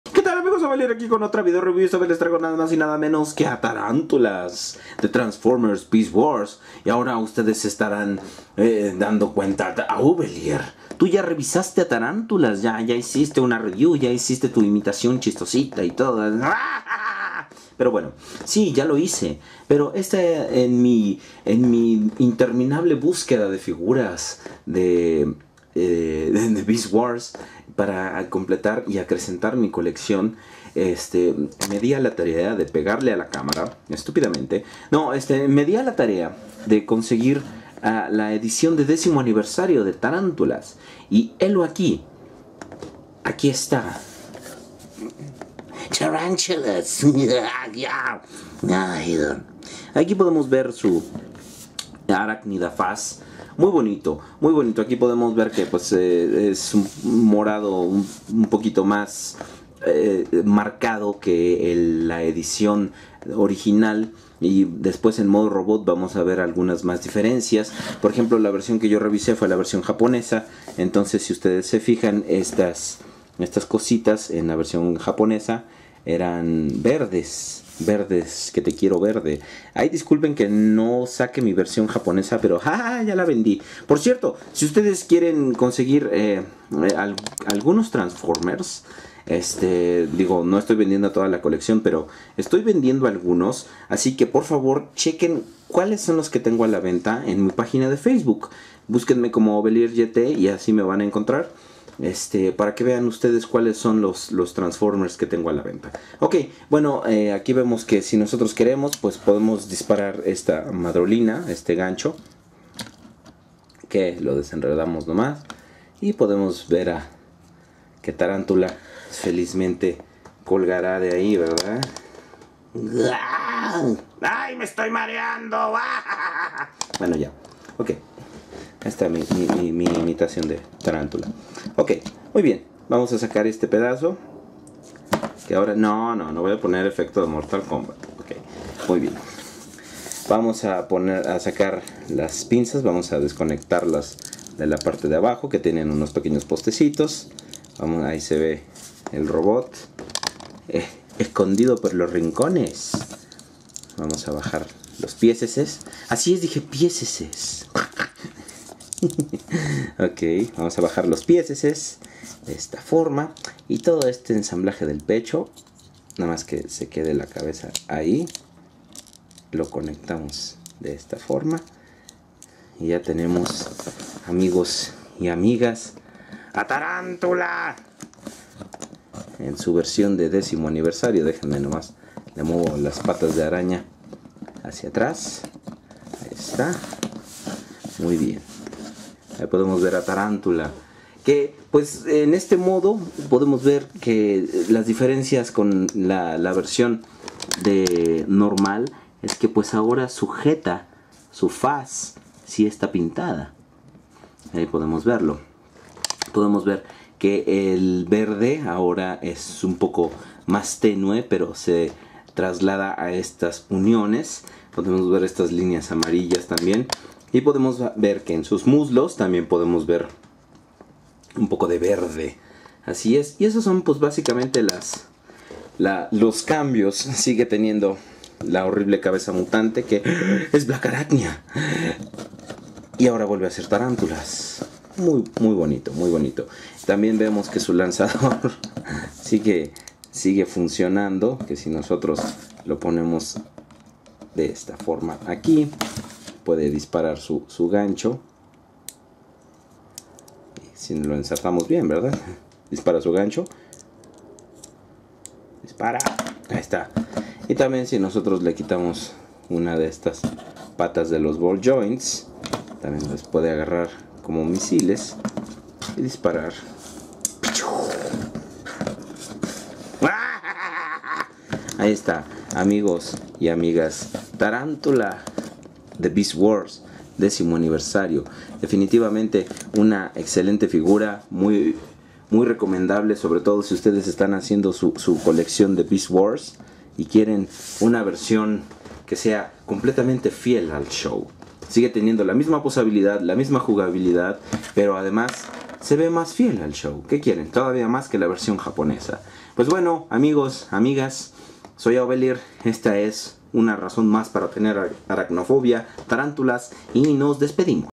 TRANSFORMERS BEAST WARS 10TH ANNIVERSARY TARANTULAS EN ESPAÑOL
La review de la razon numero uno para ser aracnofobico, que esta vez regresa con un deco mas fiel a su encarnacion en el programa... definitivamente es la versiona adquirir para los que estan haciendo su coleccion de beast wars fieles al show